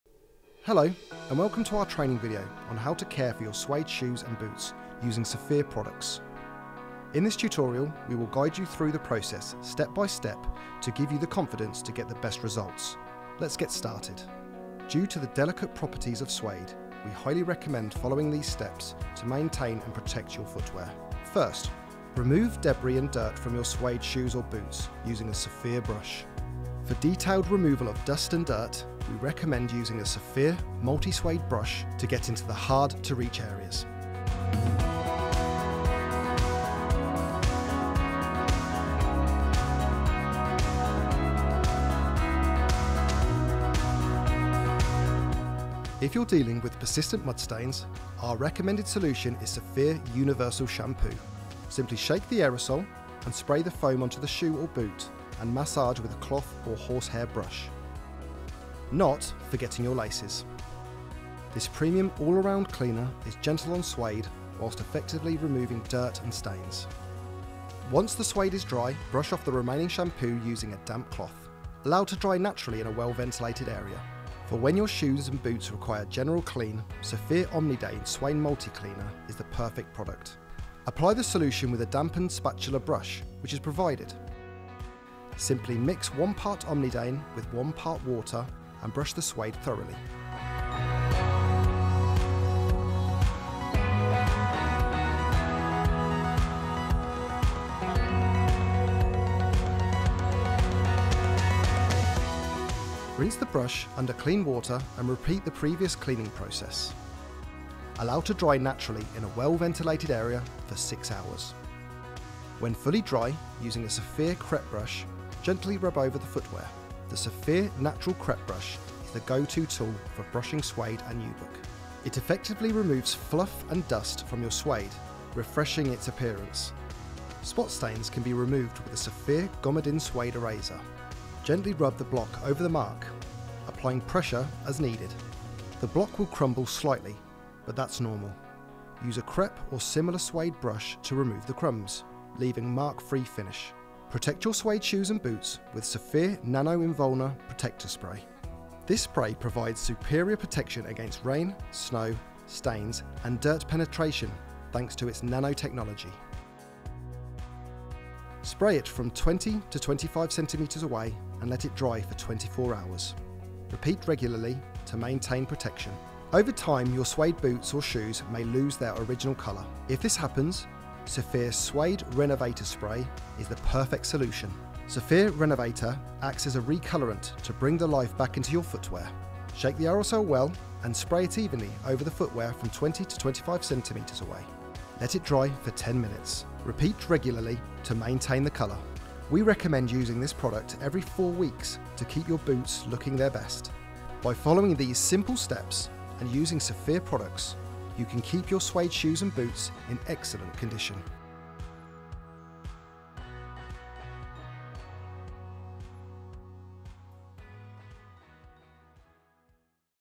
Ultimate Suede and Nubuck Boot Care: Cleaning and Protection Tips
Discover the Secrets to Suede & Nubuck Boot Care with Saphir Products! Join us in this comprehensive tutorial as we unveil the expert techniques to clean and protect your suede and nubuck boots and shoes. Saphir's premium shoe care products take center stage in this step-by-step guide. Learn how to keep your footwear looking flawless and ready for any adventure. Don't miss out on these essential care tips to extend the life of your beloved suede and nubuck footwear. Watch now and elevate your boot game!